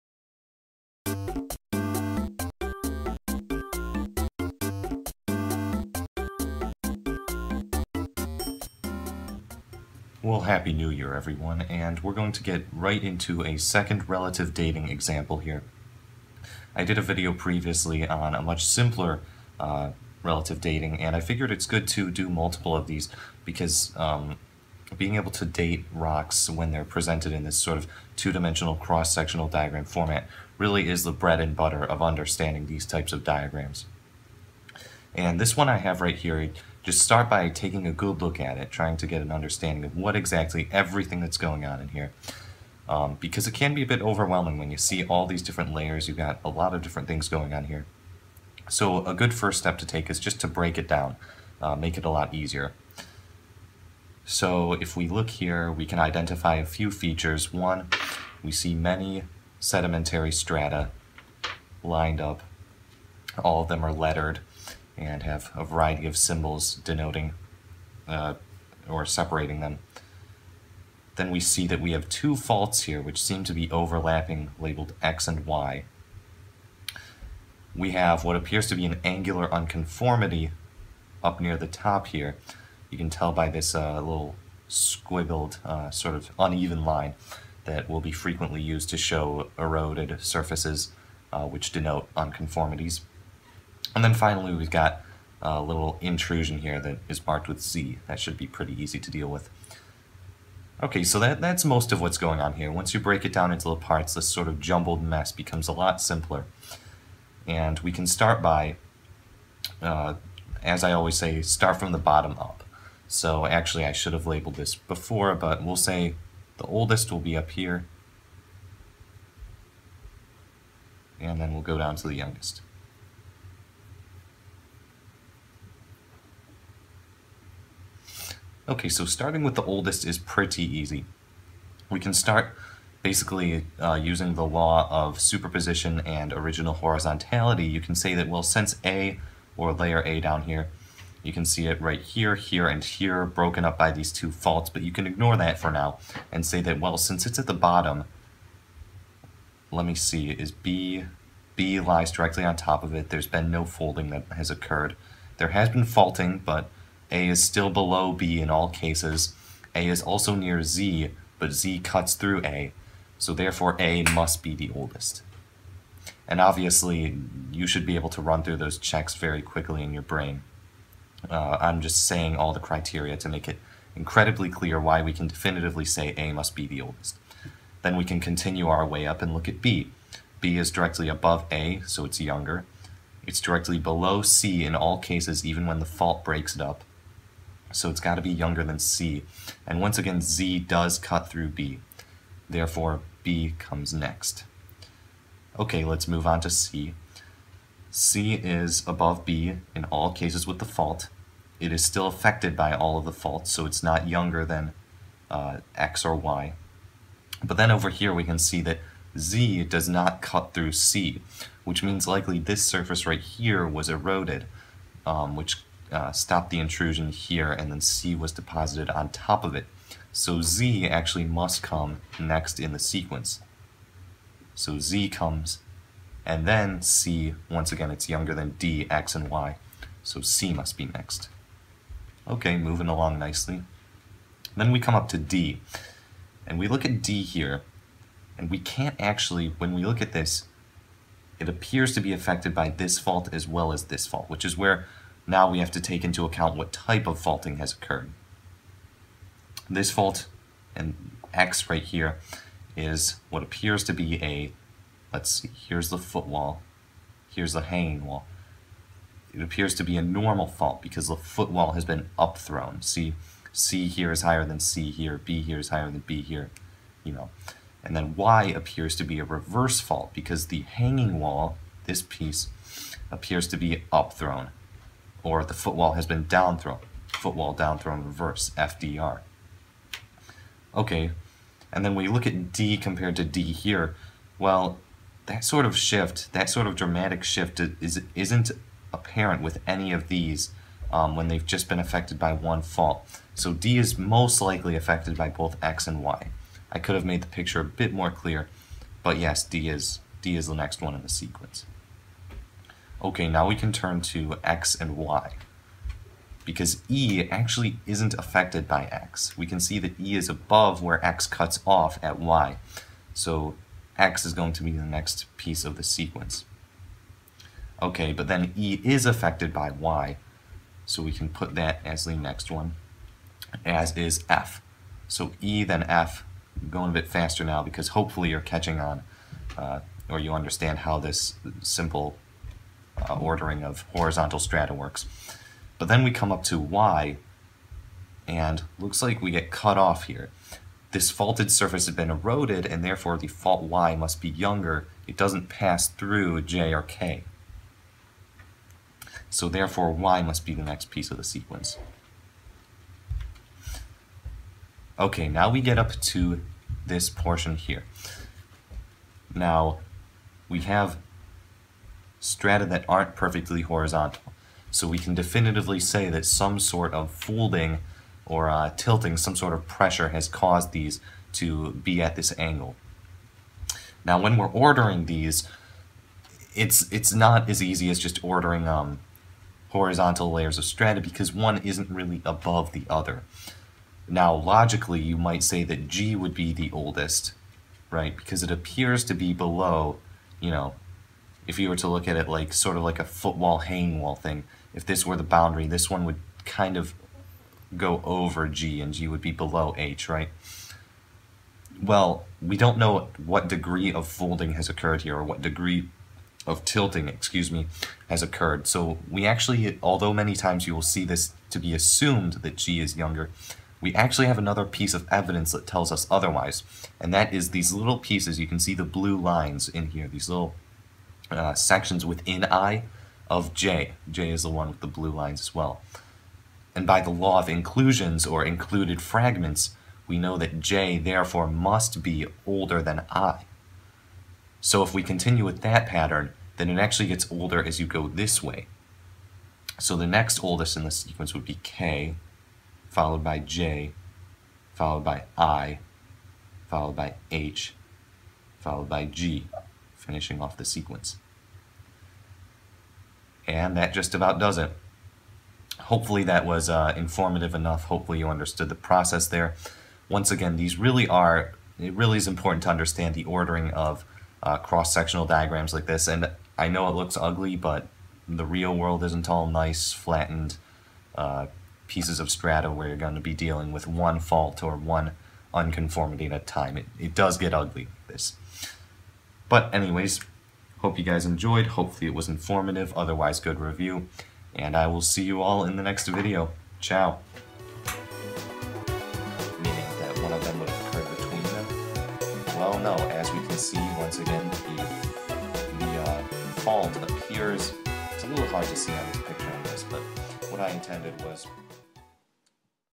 Relative Dating - Example 2
We take a look at a more complicated example of using principles of relative dating to order strata from youngest to oldest.

Also, I know it's already a couple weeks into 2017. This video was recorded on January 3rd, just a few days into the new year.